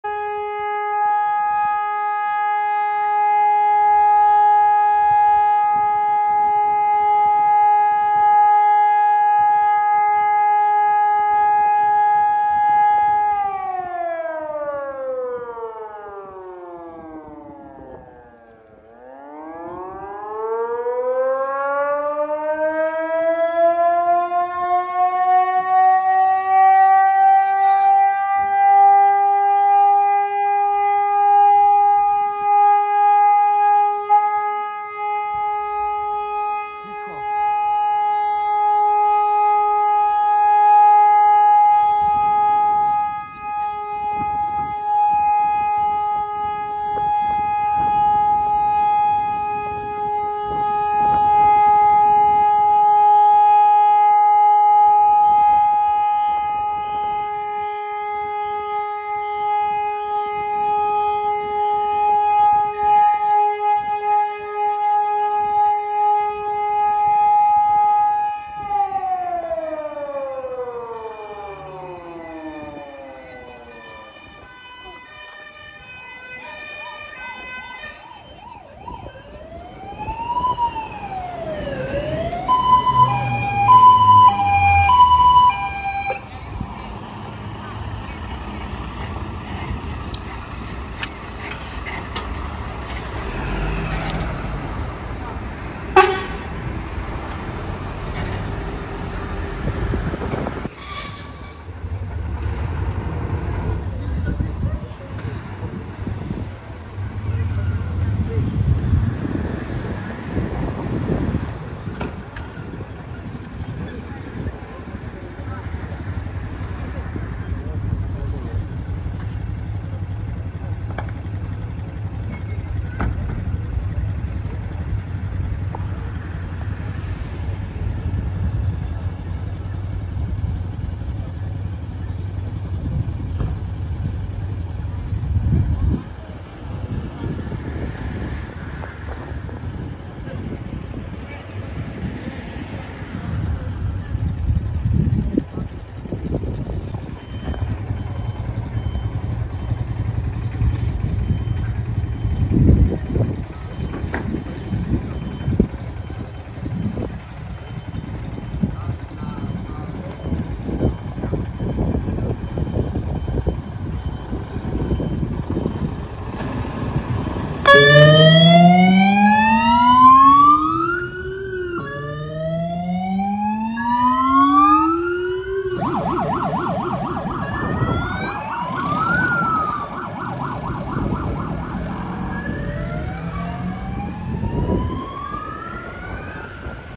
Alarmowanie, powrót GCBA i wyjazd zastępów OSP ŁEBA [4k]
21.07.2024
Alarmowanie i powrót GBA 479[G]22 z działań, oraz wyjazd 479[G]23 wraz z Quadem w tle zastęp ZRM.